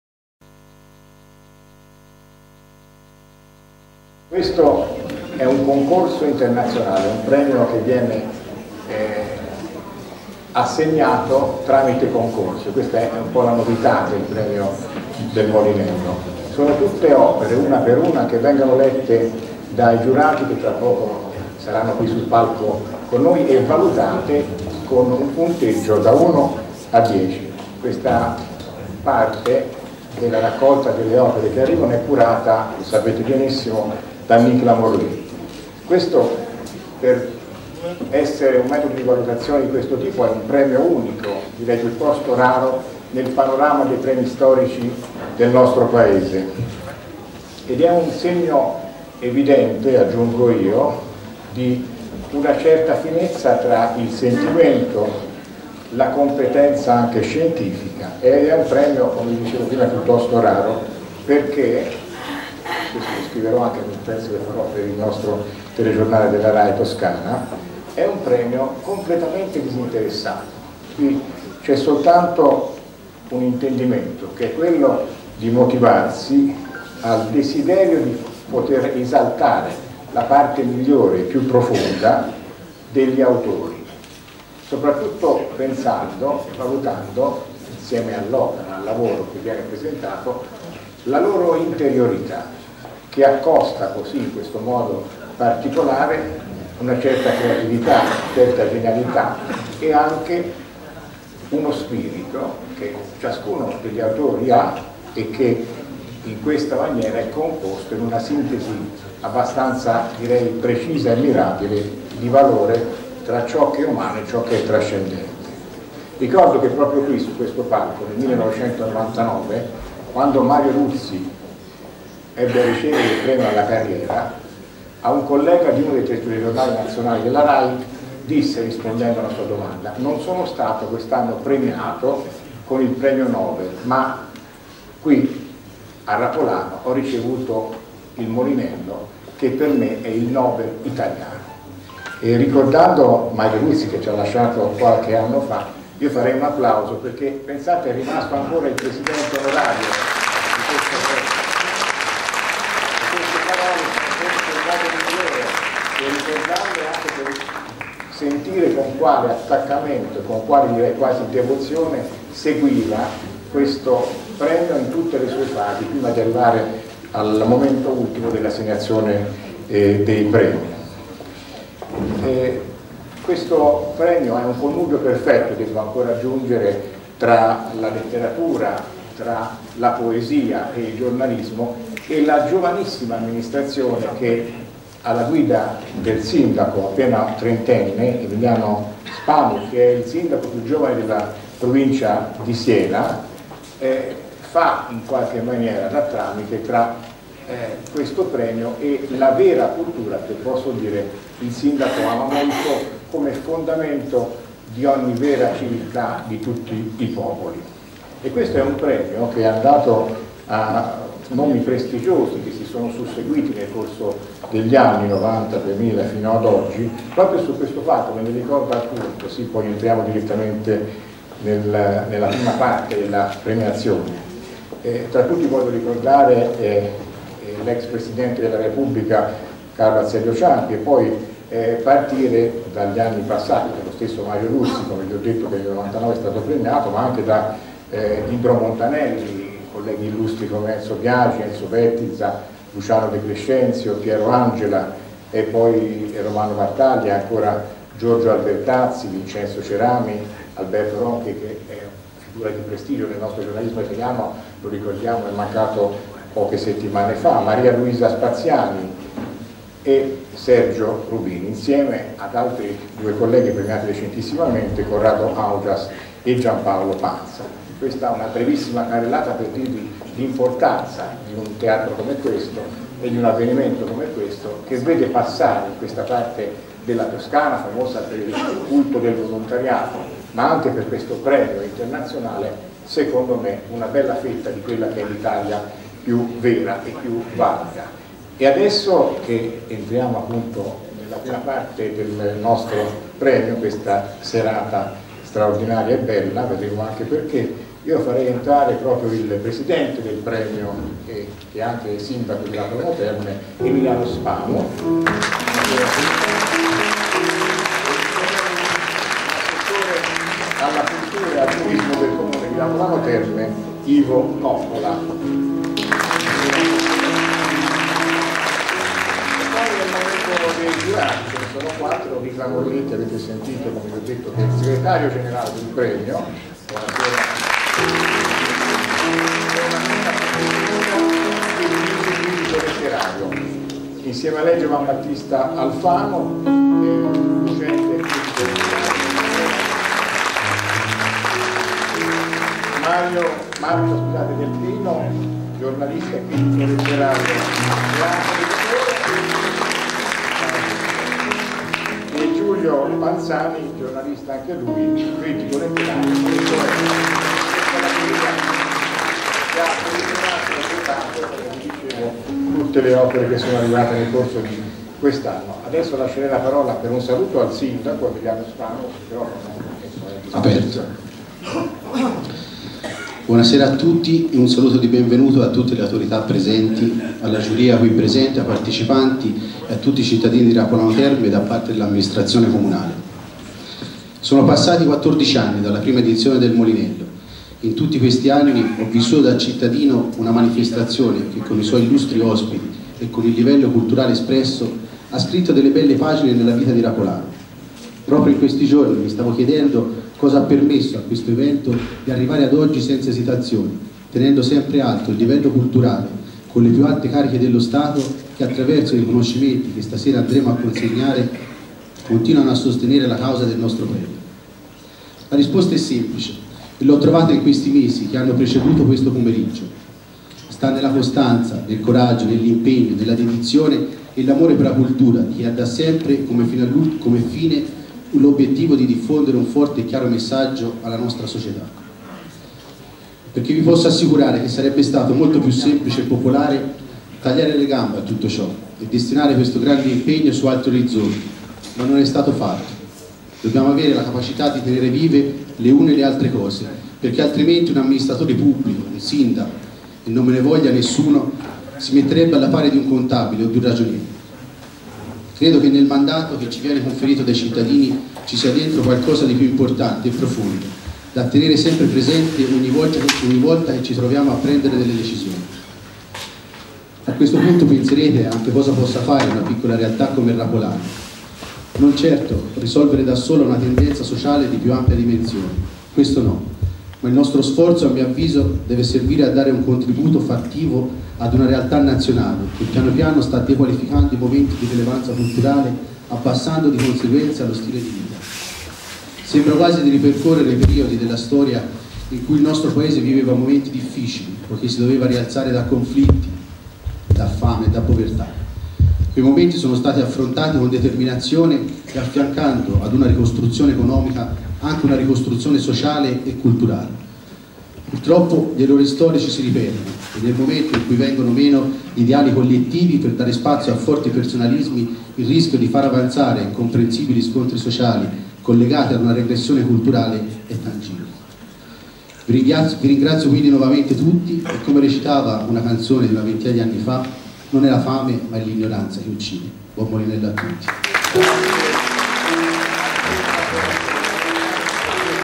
Premio Letterario Il Molinello 2010 - Introduzione
Premio Letterario Internazionale Il Molinello - Rapolano Terme (SI) - 27/03/2010, Cerimonia di Premiazione - Massimo Lucchesi, Presidente Ordine dei Giornalisti della Toscana, presenta il Premio, il segretario generale Nicla Morletti e la Giuria - Intervento del Presidente del Premio Emiliano Spanu (Sindaco di Rapolano Terme) - Video prodotto da Canale 3, adattamento web a cura di Manuale di Mari